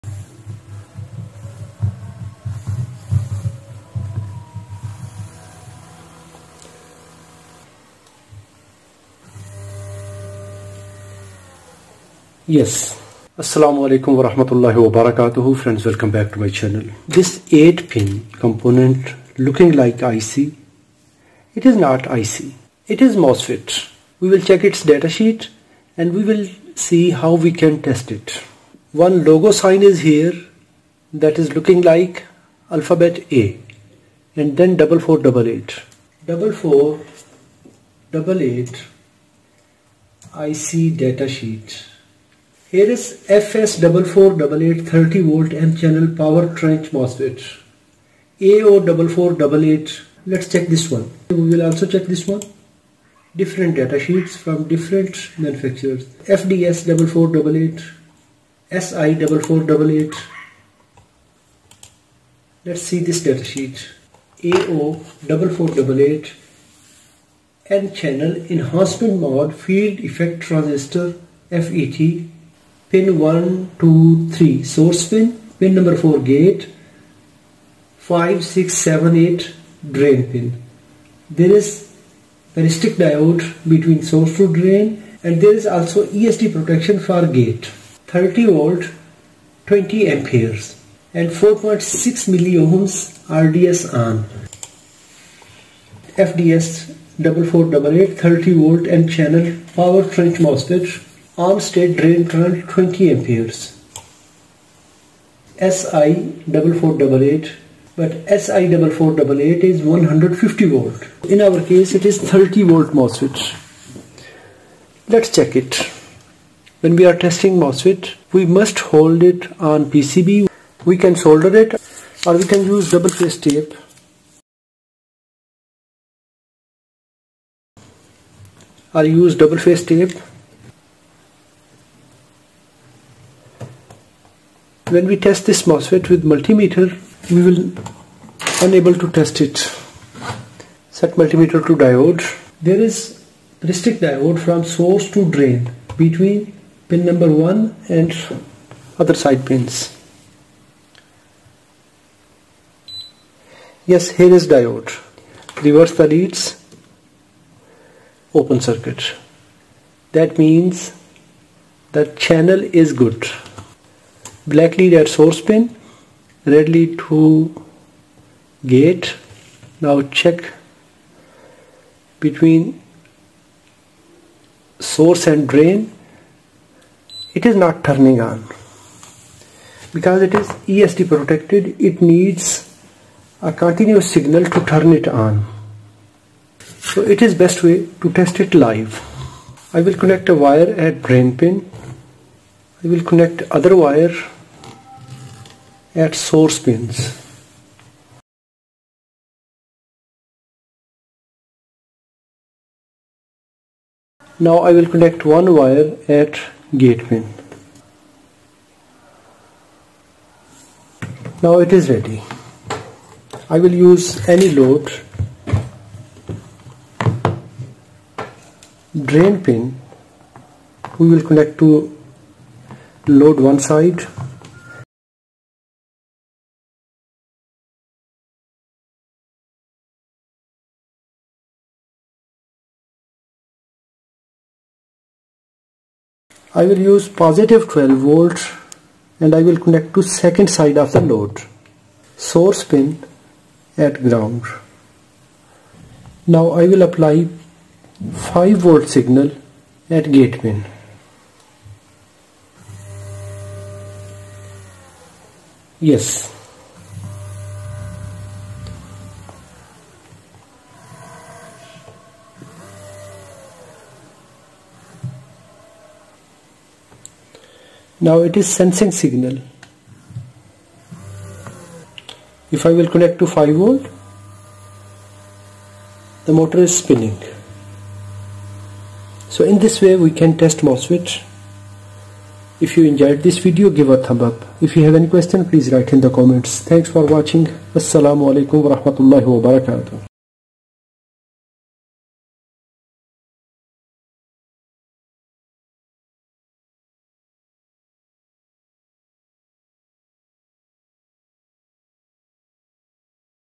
{1248} AO4488 MOSFET datasheet & testing
In this video number {1248} AO4488 MOSFET datasheet & testing. I explained how to find AO4488 / FDS4488 soic-8 soic8 mosfet. it is 8 pin smd mosfet. i demonstrated practical testing of mosfet using multimeter and live testing